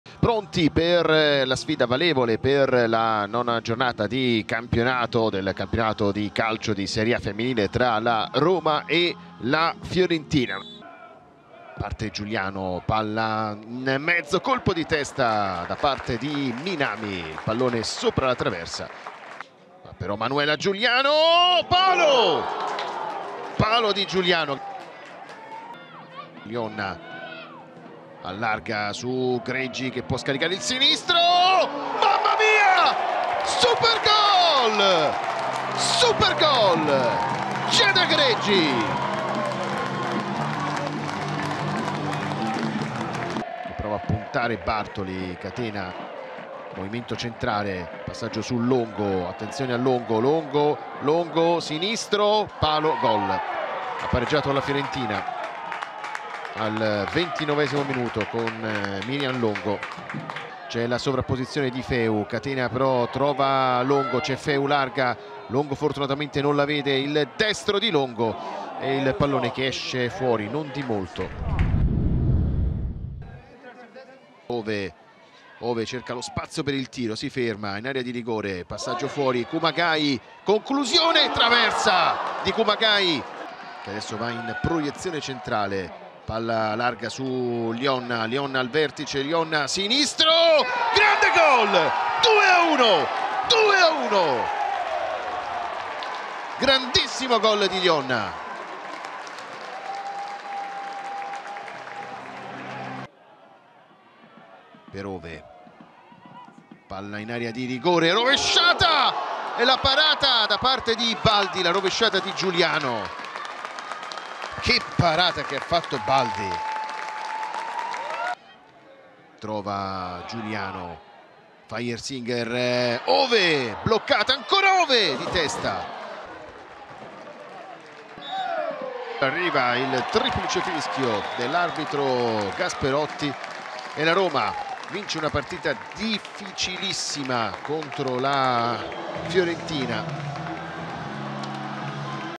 ❤️‍🔥 Un’altra grande vittoria! 🙌 Roma 2-1 Fiorentina | HIGHLIGHTS SERIE A FEMMINILE
⚽️ 2-1 con i grandi gol di Giada Greggi e Benedetta Glionna 👊